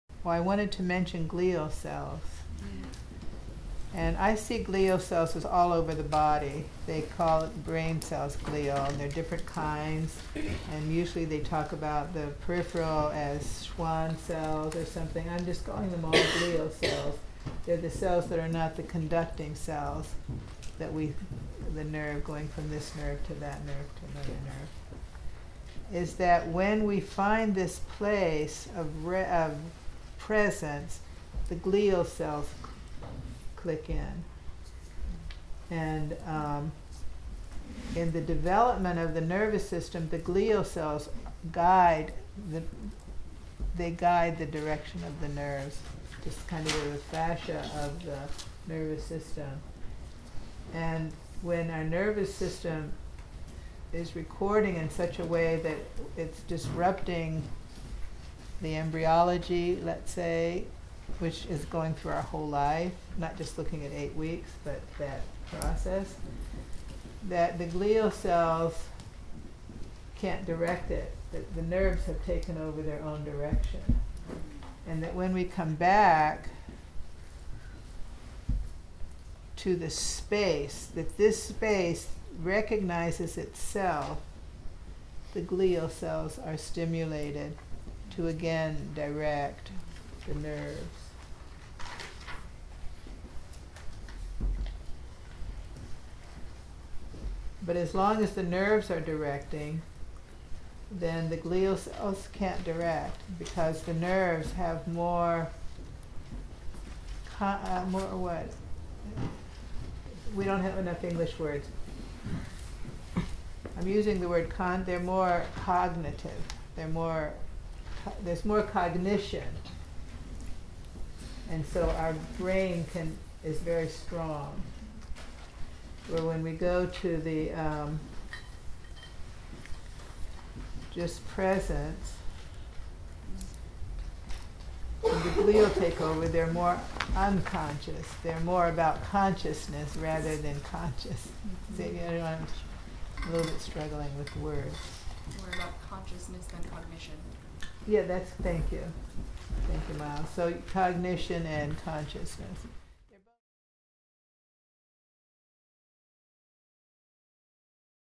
Glial Cells and the Nervous System
Glial cells are the foundational support for the nervous system. In the embryological development of the nervous system, the glial cells guide the direction of the conducting nerves. I view our embryological process as not just the first 8-weeks but as a process that continues throughout our whole lives.

When our conducting nerves are transmitting in a way that disrupts our embryological process, they lose the underlying support of the glial cells. The nerves have taken oven their own direction.

When we come back to the space of the structure, rather than the solidity of the structure, the space recognizes itself and the glial cells are stimulated to again support the direction of the nerves.

This clip is from the online course Embodying the Embryological Foundations of Movement: A Body-Mind Centering® Approach.